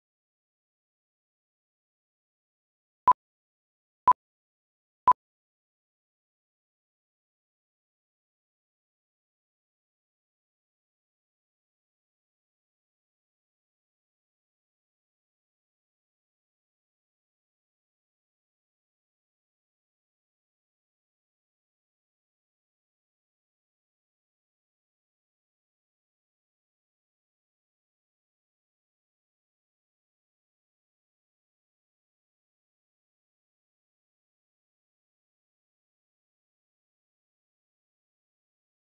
CLIVE AUDITION AUTO-CUE
Hi I'm CLIVE and
welcome to my Youtube
audition.

The CLIVE Project is a
joint venture between
digital media specialists
award winning
corporate video
production company
ridgefilms.com.au.

CLIVE is an easy to add,
transparent video layer
of professional
presenters, which
significantly increases
your web sites
effectiveness and online
customer engagement.

If you like my audition
then vote for me and
check out
CLIVEVideo.com for
more information.